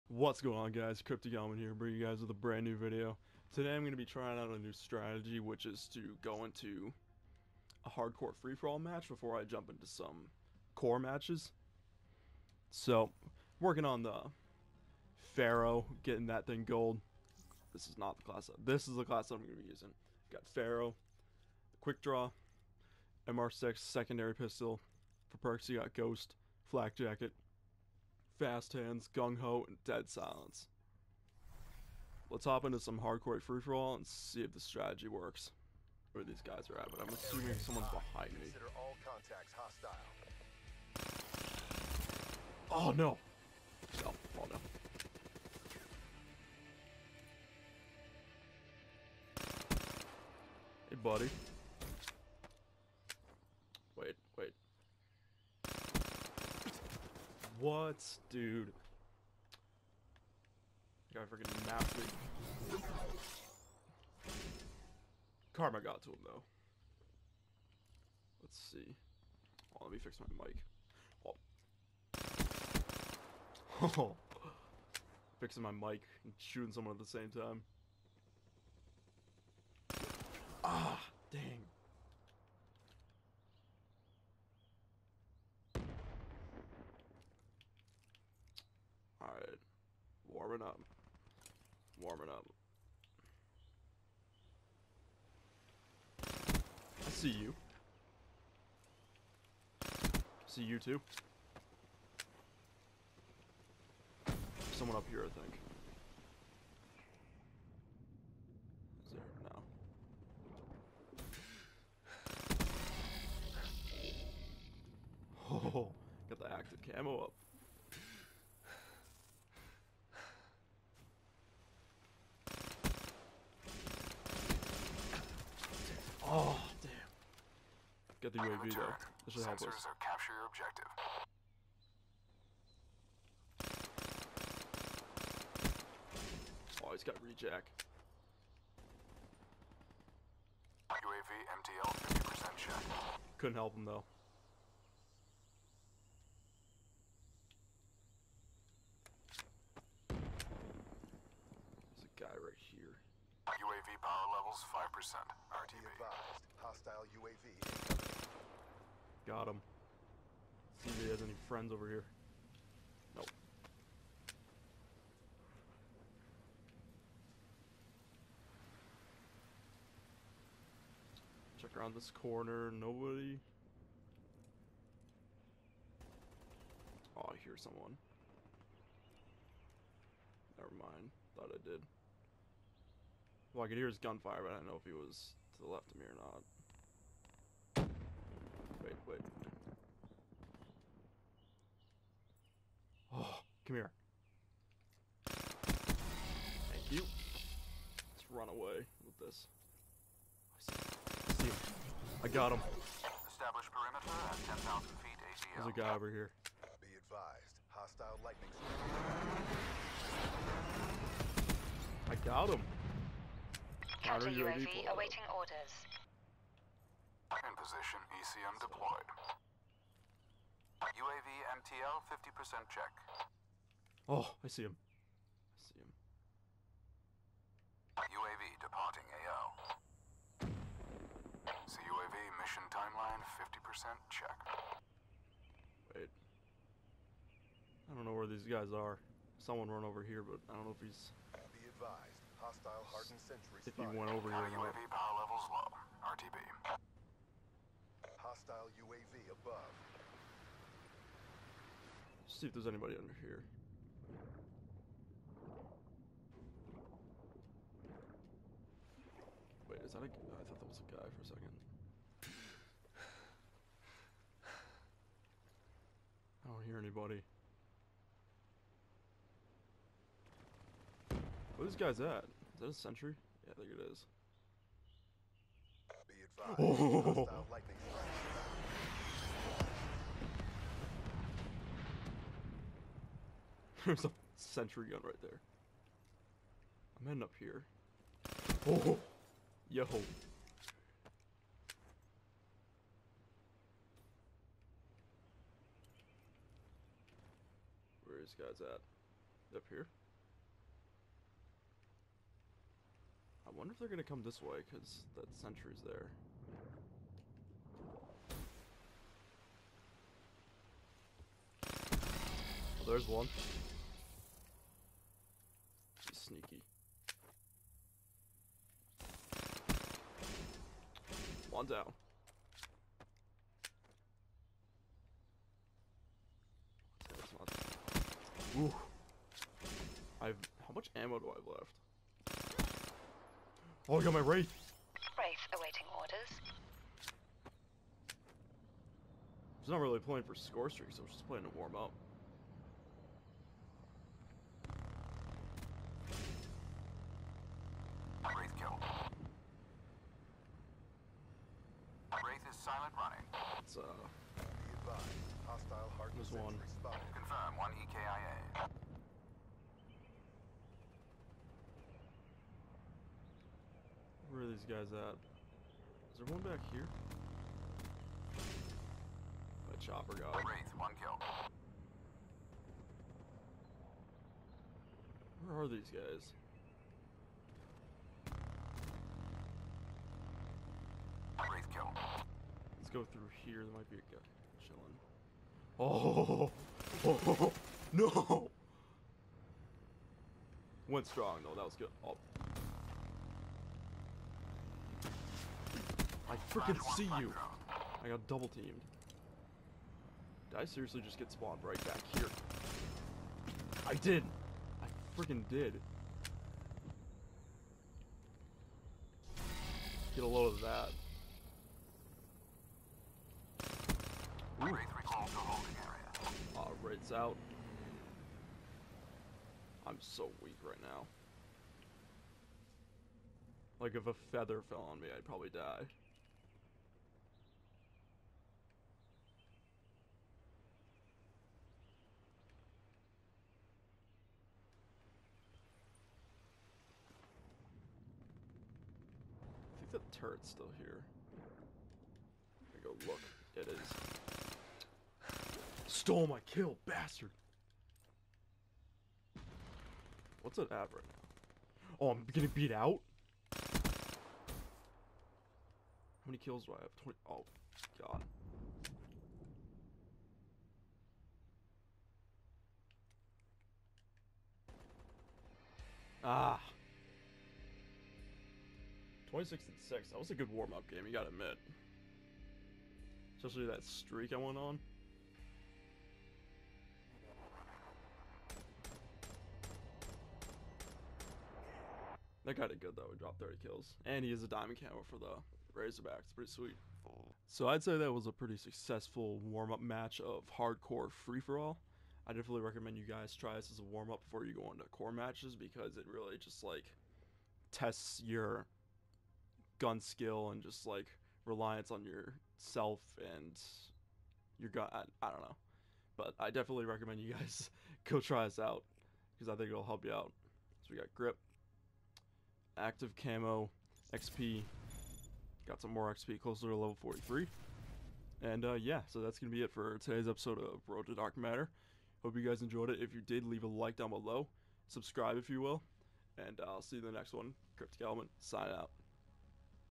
NEW STRATEGY! (Road to Dark Matter EP. 5)
Working towards the DARK MATTER camo by playing some HARDCORE FFA! Leave a "LIKE" if you enjoyed the video! :)